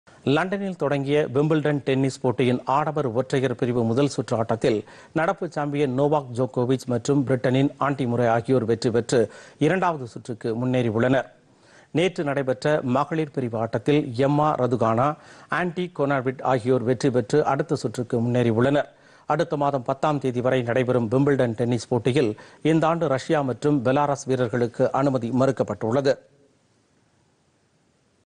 விம்பிள்டன் டென்னிஸ் போட்டி
விம்பிள்டன் டென்னிஸ் போட்டி - ஆன்டி முர்ரே - நோவாக் ஜோகோவிச்  இரண்டாம் சுற்றுக்கு முன்னேற்றம்